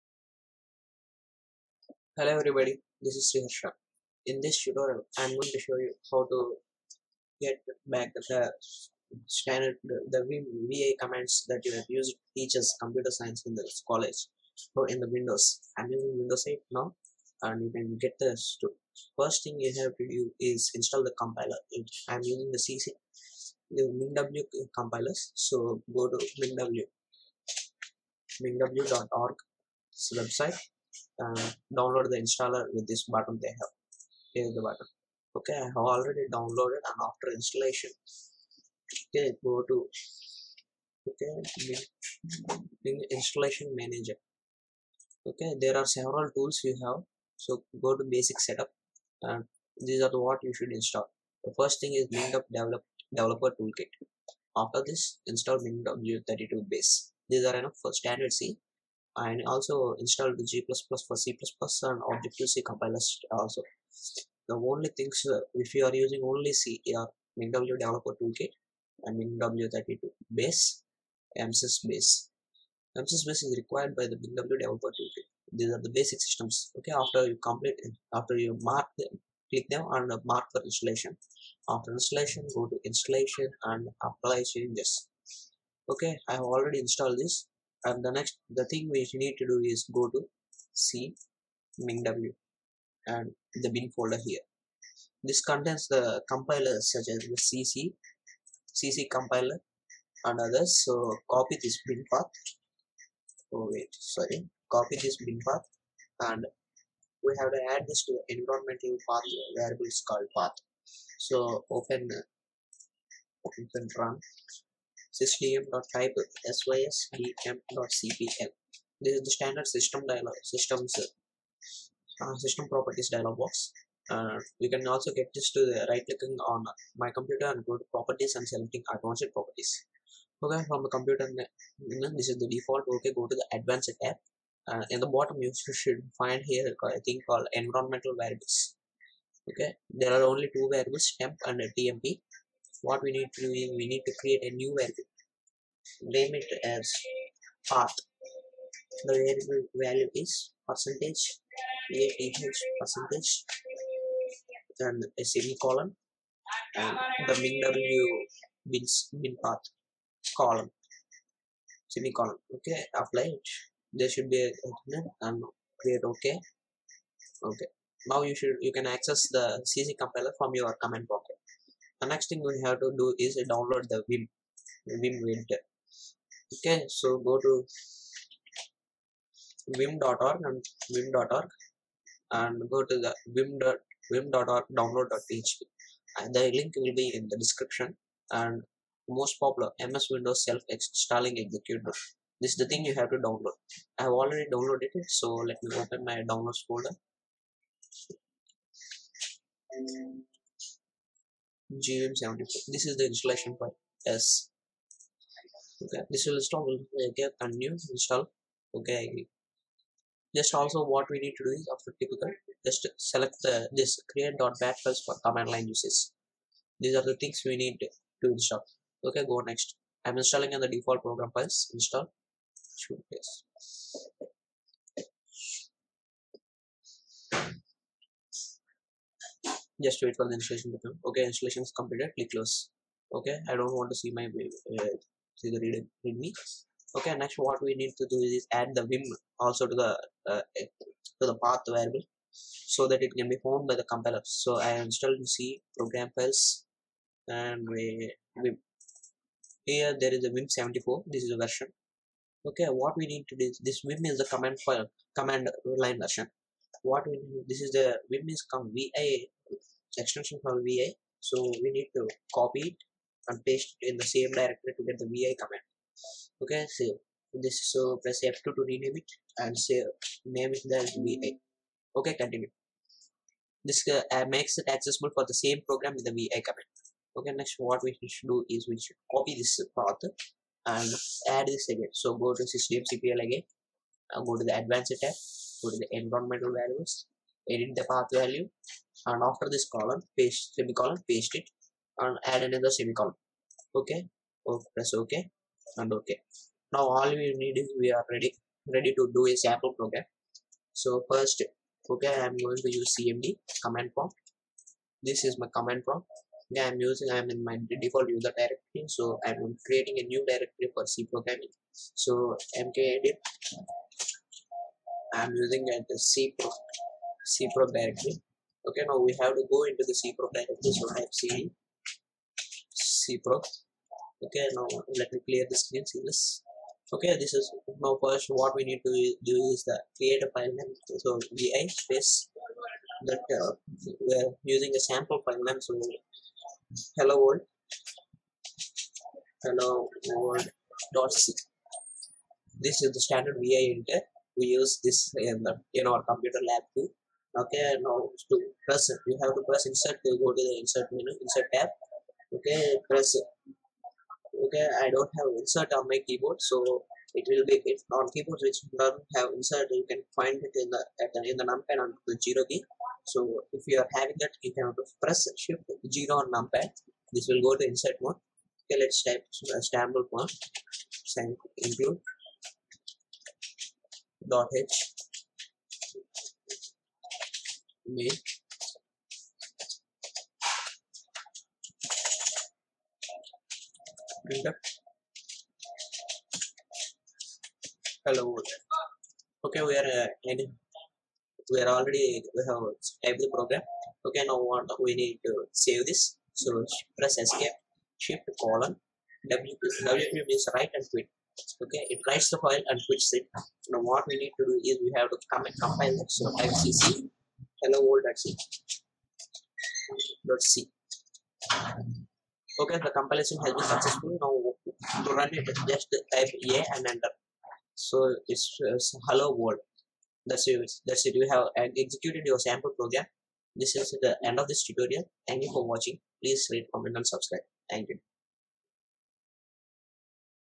Get VI Like Linux Commands In Windows
In this tutorial, I'm going to show how to get the VI like commands used in our CSE lab in windows. The software we use is

Just follow the instructions and you should be done.